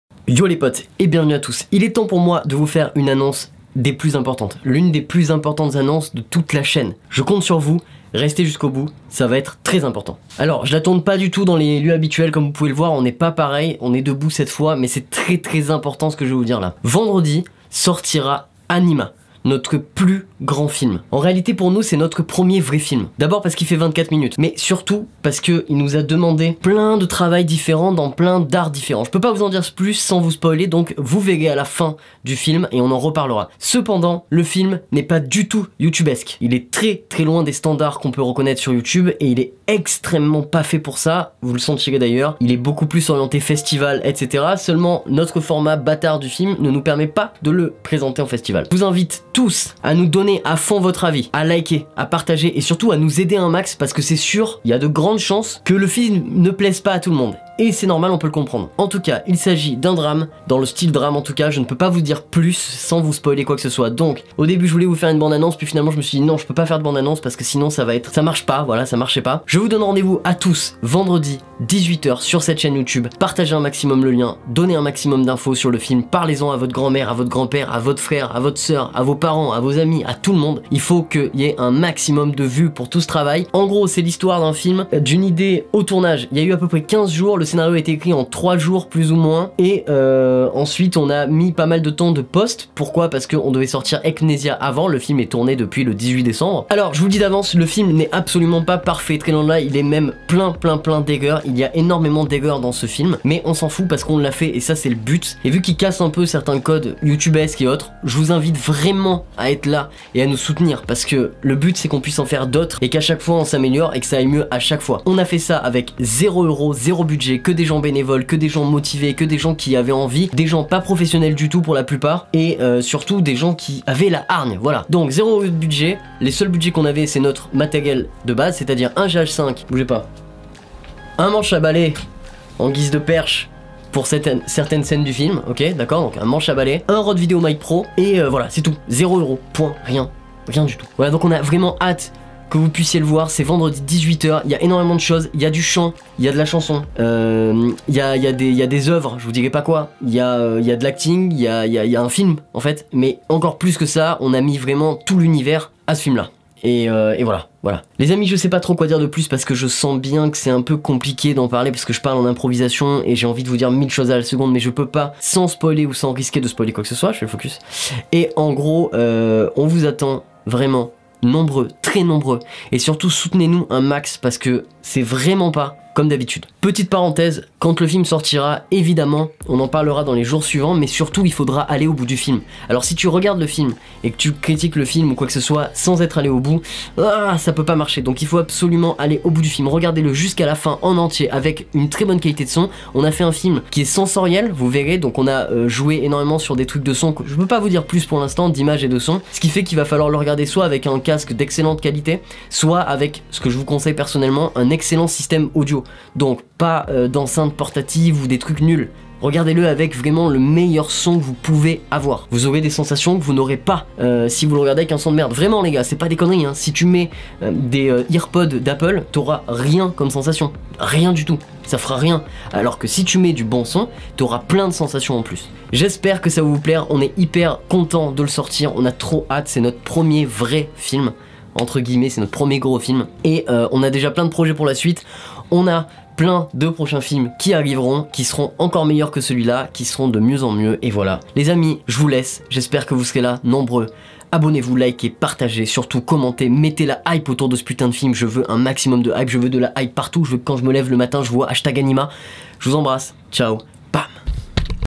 GROSSE ANNONCE !!! ANIMA VENDREDI 18 H
Discord (Viens on est bien)
LISEZ LA DESCRIPTION LES COPAINS

Me soutenir : En cours de construction

Aujourd'hui les potes une annonce importante, notre prochain film Anima sera disponible vendredi à 18 H on vous attends très nombreux et surtout n'hésitez pas à liker !